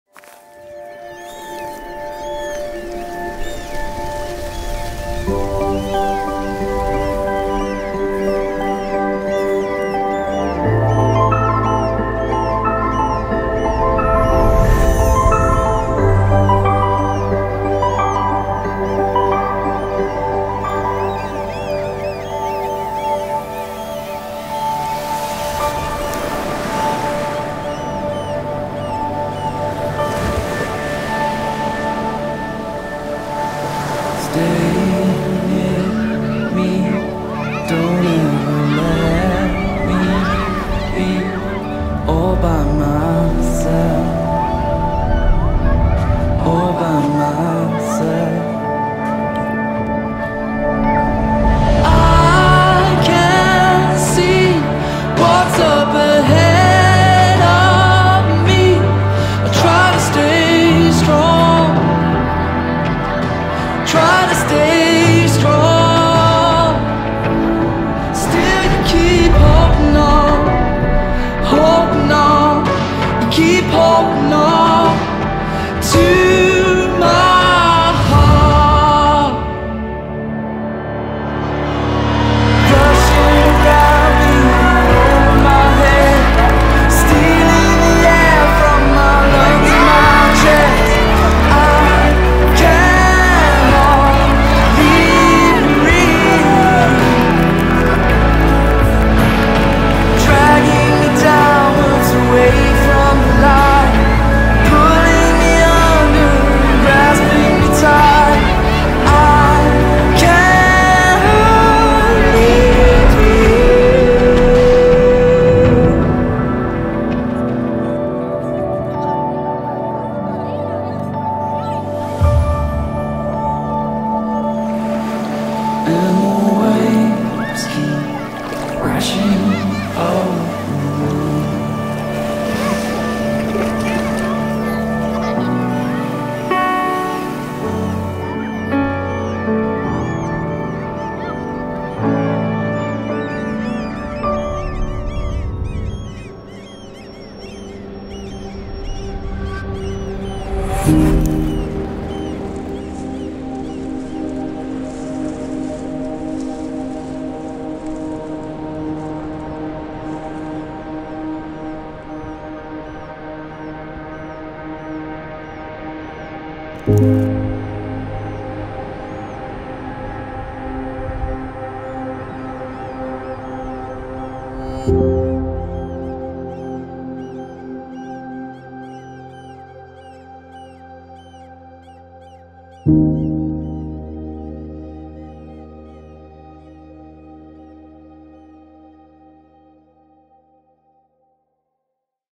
Dänemark 2024 | HSV-Leichtathletik
Dänemarkfahrt nach Bjerregard vom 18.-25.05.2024